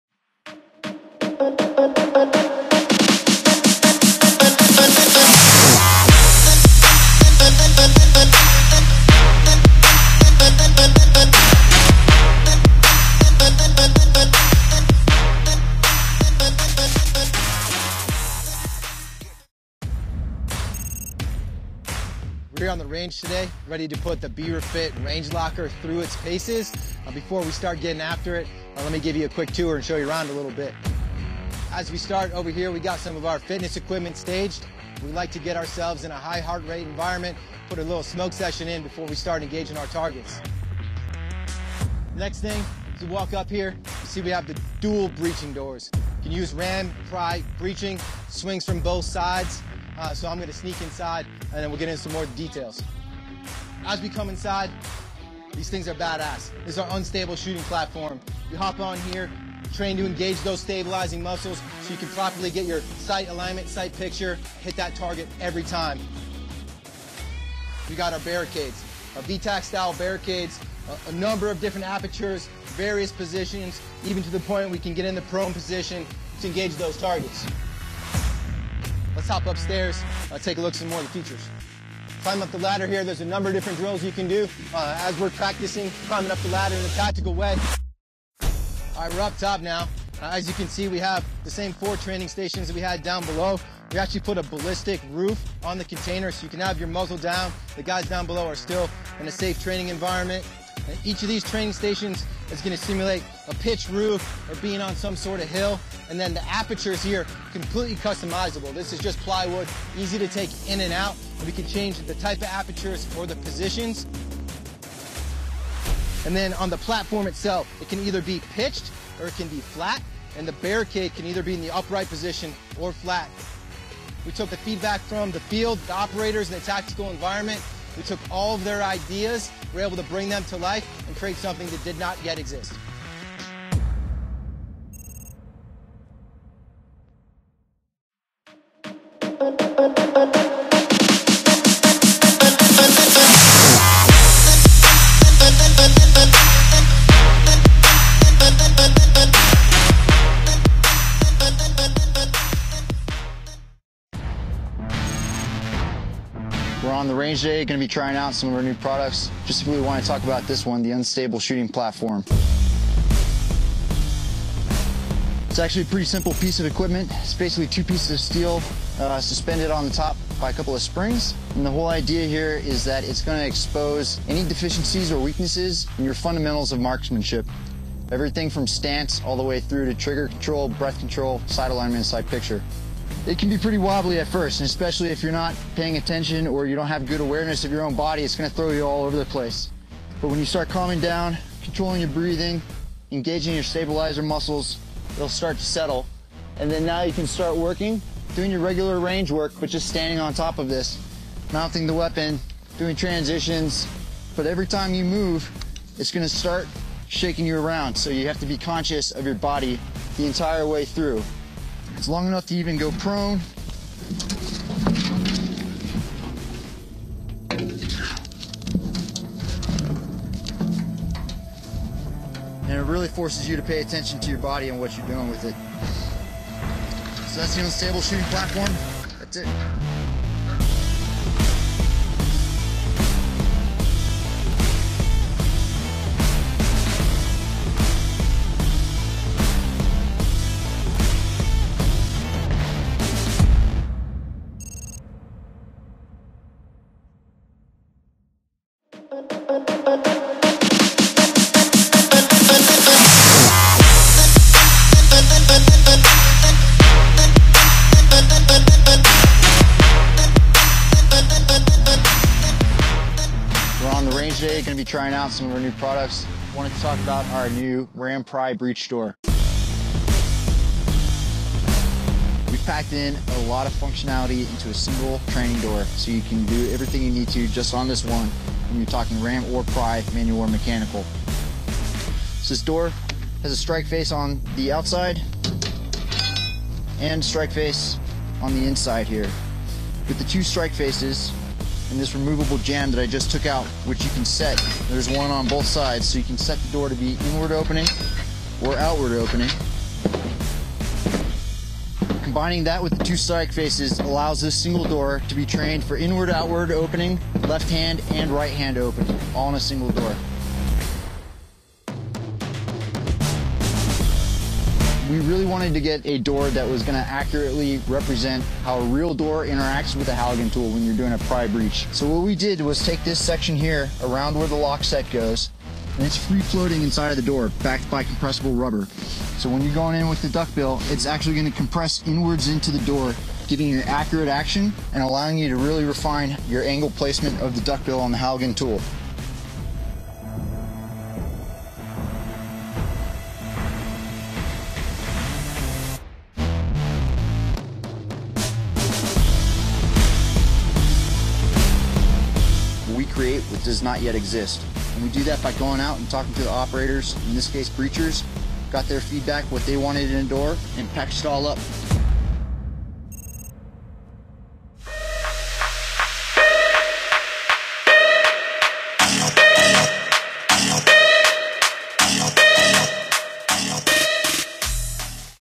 BeaverFit Range Locker - ADS Virtual Warfighter Expo Demo
This video was presented by Beaverfit at the ADS Inc Virtual Warfighter Expo to showcase their new Range Locker system.